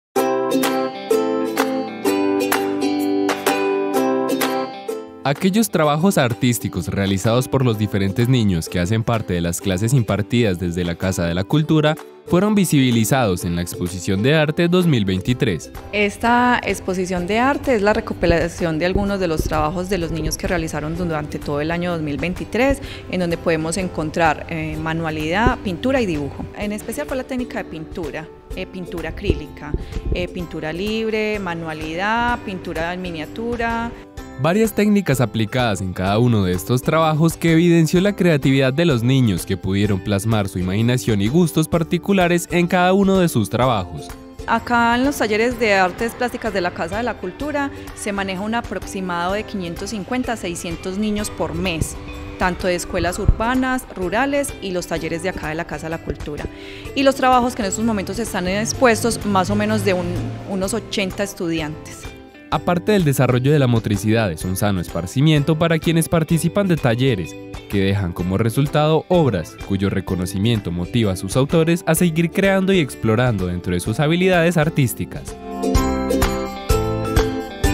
Exposición de arte 2023
Son varias las técnicas artísticas que se enseñan desde los talleres en la casa de la cultura y, a través de la exposición de arte 2023, se dejó en evidencia lo aprendido en estas clases por los niños que aprendieron y desarrollaron sus capacidades a través de las mismas